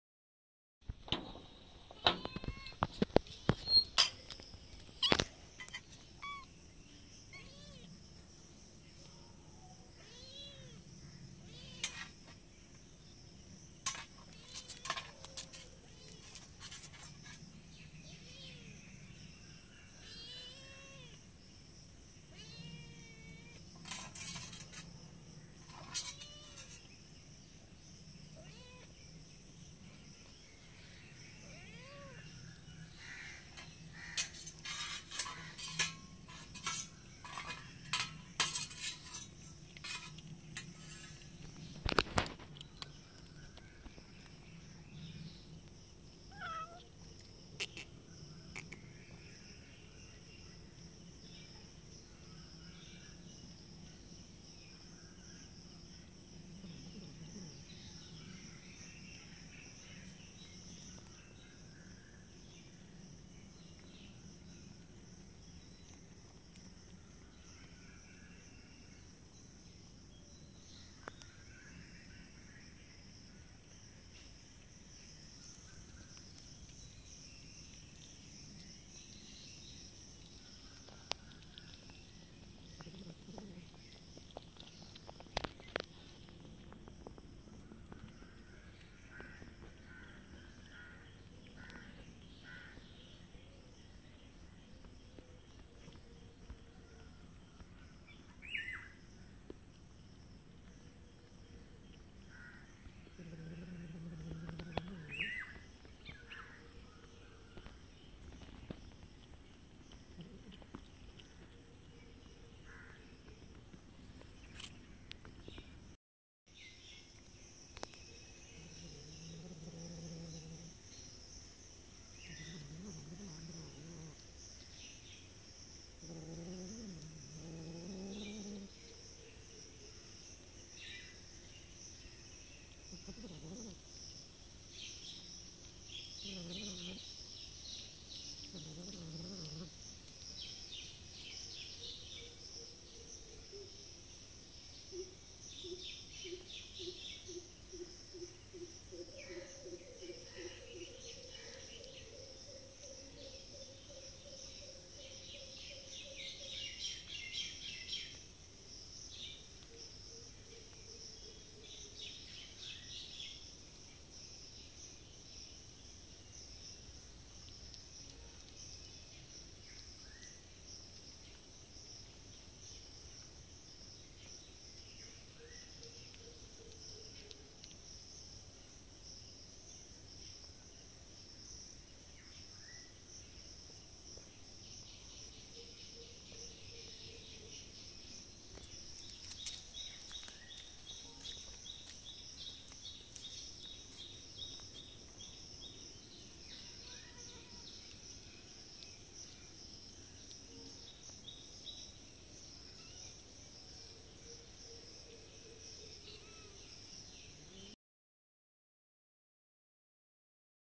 Hungry Cat Family.
Hi friends she today also hungry Cat family knocking my door for asking food they are so beautiful. I gave them Rice with Fish.